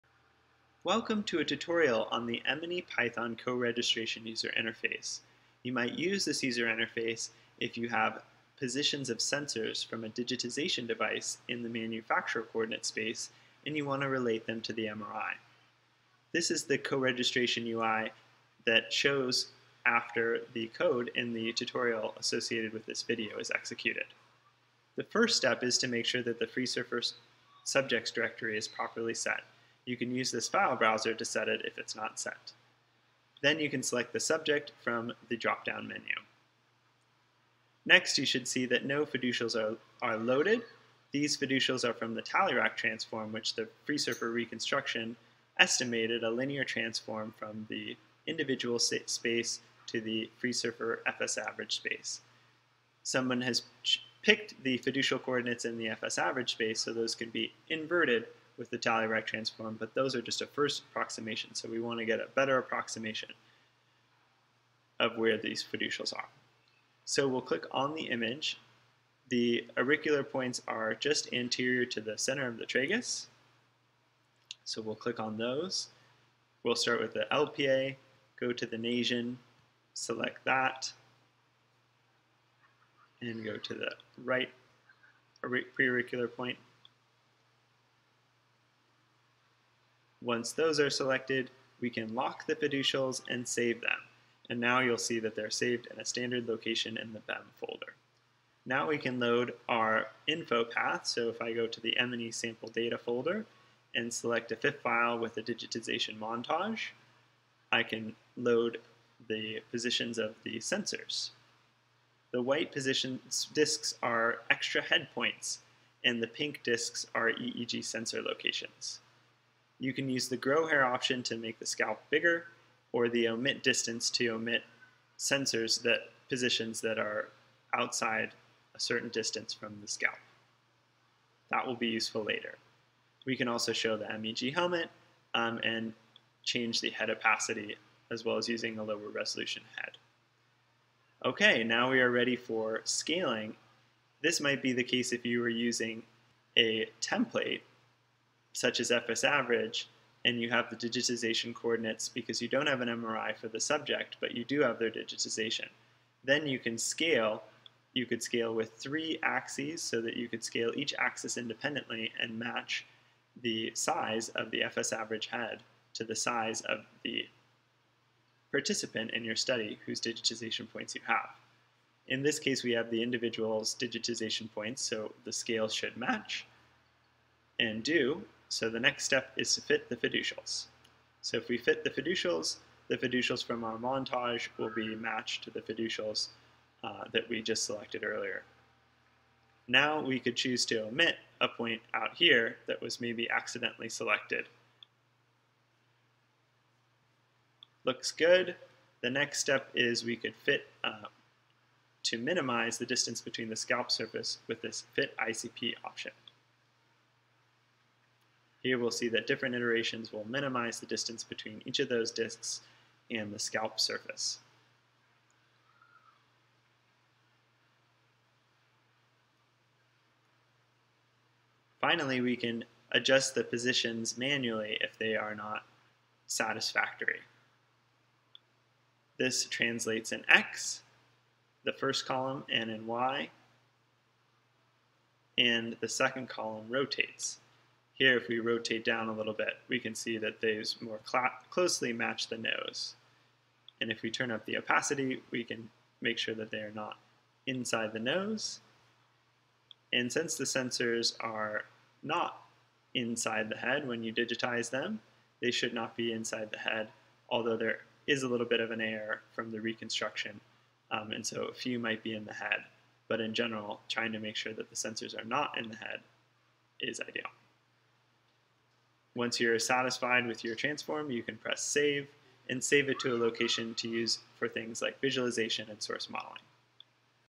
MNE-Python Tutorial on Coregistering EEG Digitization Points with an MRI
This video accompanies this tutorial The goal is to take digitization points from a Polhemus Pen, bifocal camera digitization (e.g. Localite sold with Brainvision products), geodesic dome or other digitization equipment and transform them from their native space (defined by the manufacturer) to align with a head model in magnetic resonance (MR) image space. How to load in the data and the options for automated vs manual fitting are detailed. This process allows you to associate sensor positions such as EEG or functional near-infrared spectroscopy (fNIRS) with the brain of an individual participant based on their MR image in order to estimate brain activity from the sensors more accurately than just using standard positions of sensors on a template brain.